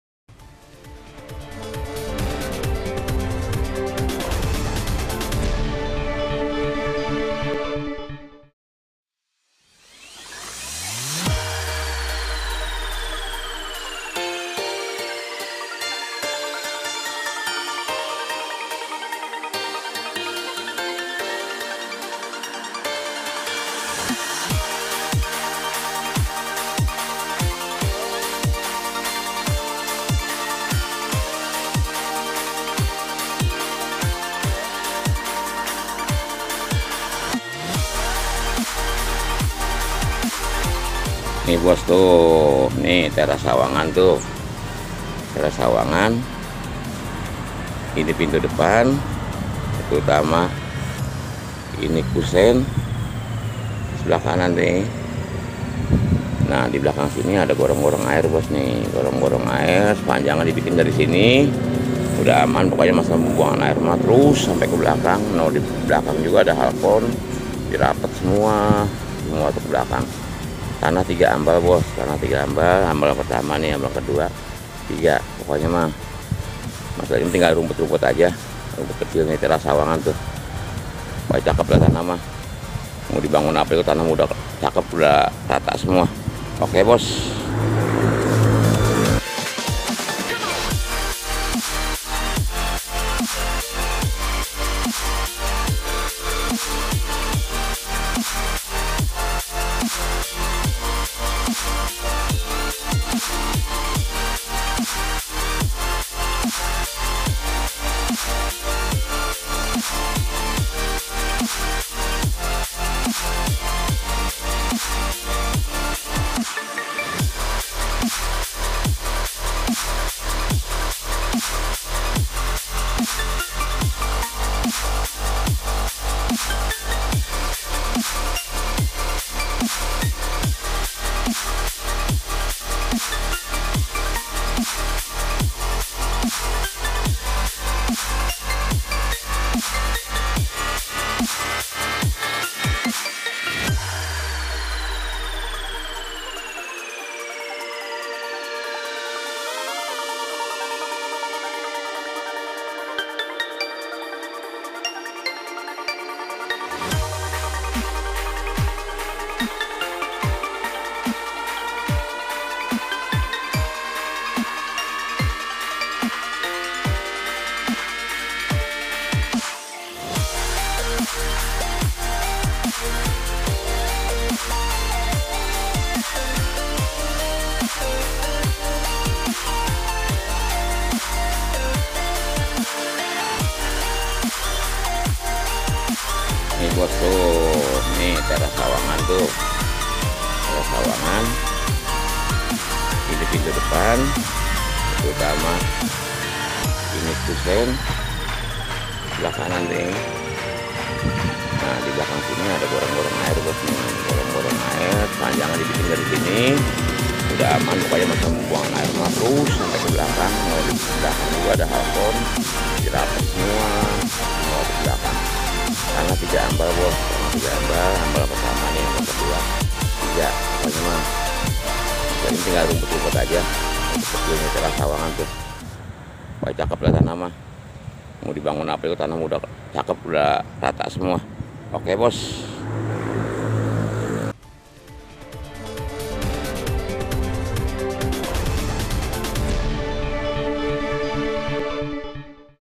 Tanah Dijual luas 4500 m2 di jalan raya pengasinan Sawangan depok samping Teras Resto
Tanah luas 4500 m2 lebar muka 20 m lokasi akses masuk mobil di jalan raya pengasinan sawangan depok dekat teras sawanagn dijual murah aja per m 2,5 juta surat shm, bisa dibangun  rumah atau kontrakan atau gudang. investasi tanah menguntungkan di depok.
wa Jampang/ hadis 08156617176